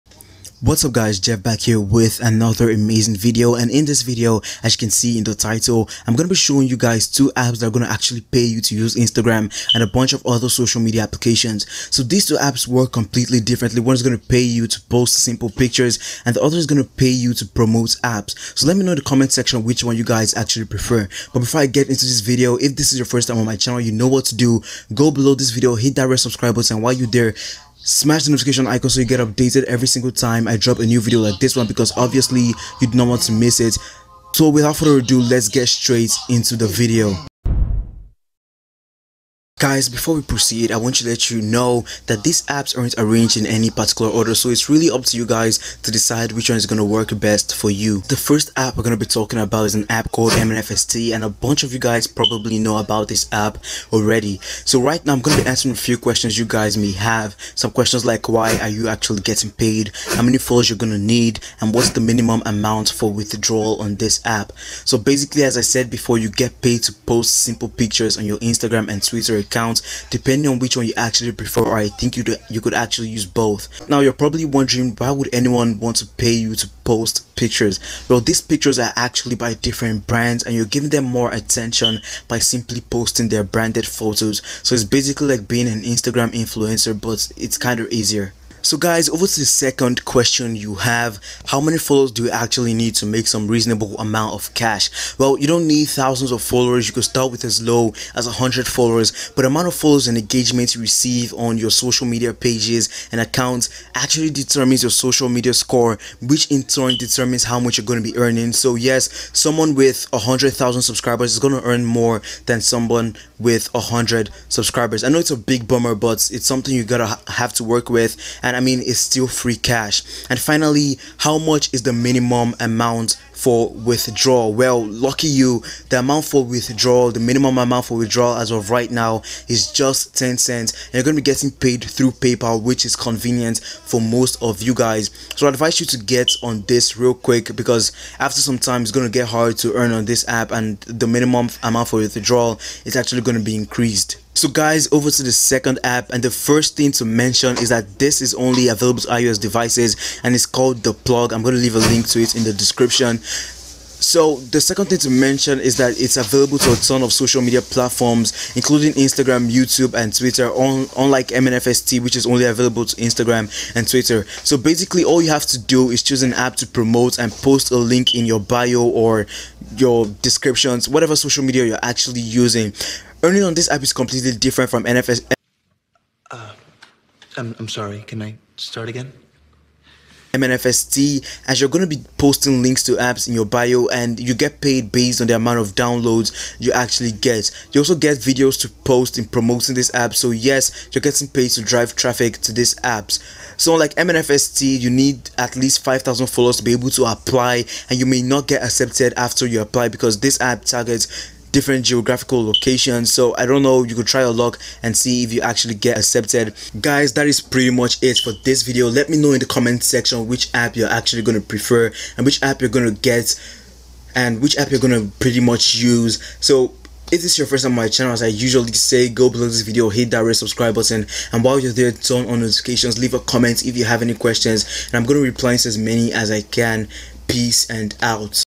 Make Money On Instagram Posting Pictures | Mnfst vs The Plug
Make money on Instagram with this two apps Mnfst and The plug.
In this video i explained how to make money on instagram with two apps that will pay you to use instagram, without thousands of followers. And you do not need to be an influencer, the first app will pay you to post pictures and the second pays you to promote apps. So its an easier method of monetizing your instagram account and you do not need traffic for the first app.
Mnfst and The plug

If you have any other methods on how to make money on instagram share in the comments section.